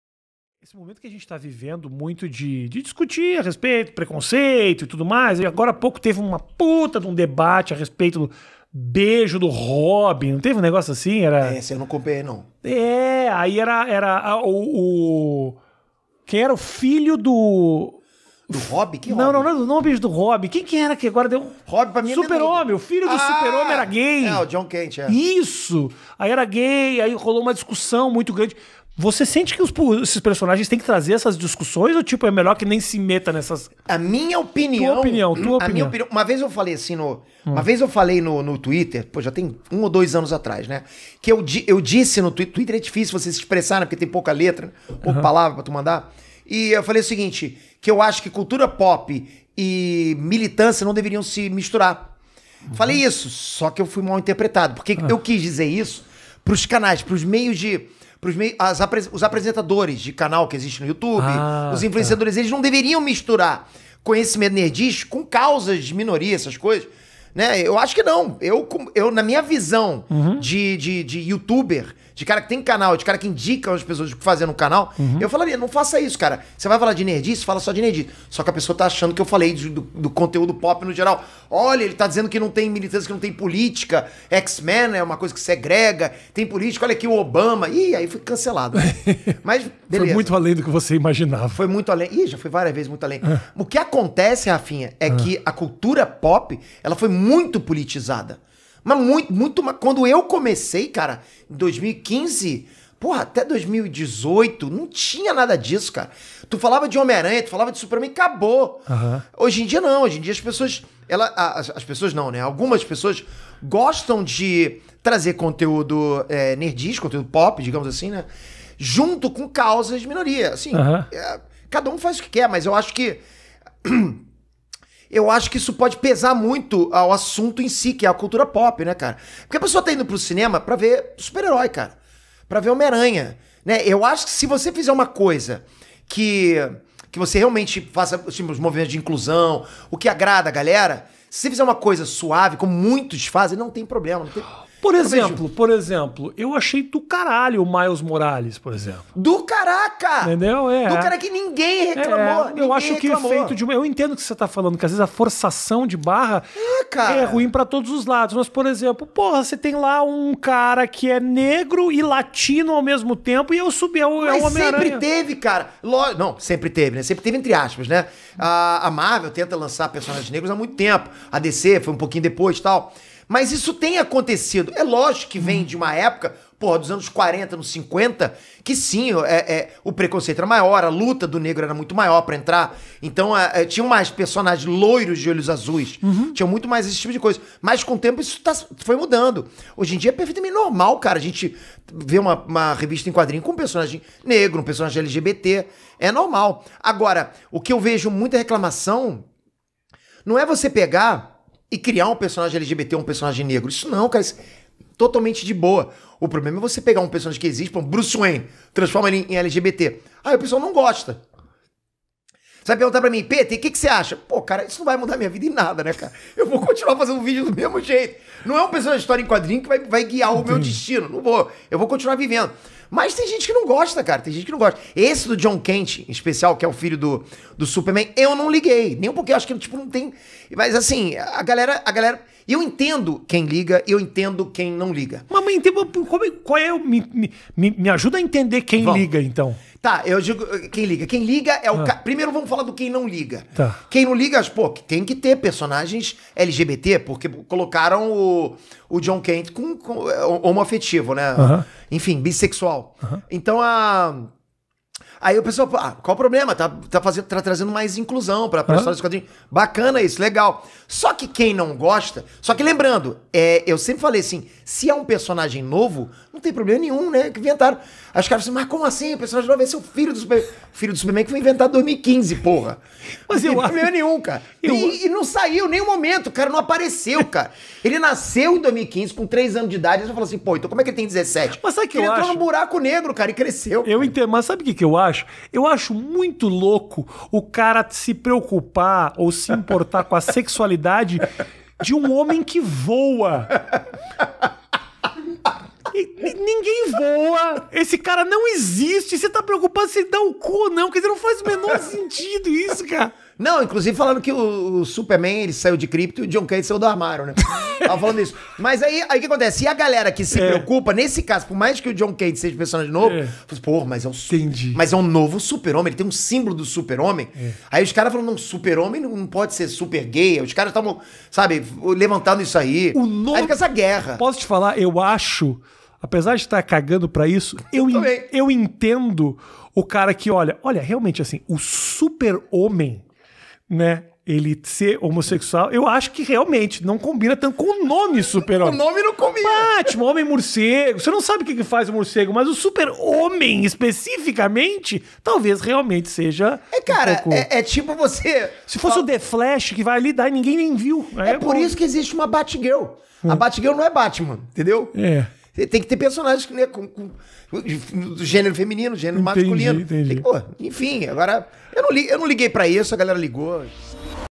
MILITÂNCIA NA CULTURA NERD - PETER JORDAN
Peter Jordan é apresentador do Ei Nerd, posta vídeos de reacts, vlogs e bastidores de filmagens no canal Peter Aqui e é criador do Acredite ou não. Além de youtuber, Peter é empresário e dono da Petaxxon, a desenvolvedora que criou os sites Letras e Cifras e tem sede no Brasil e Estados Unidos.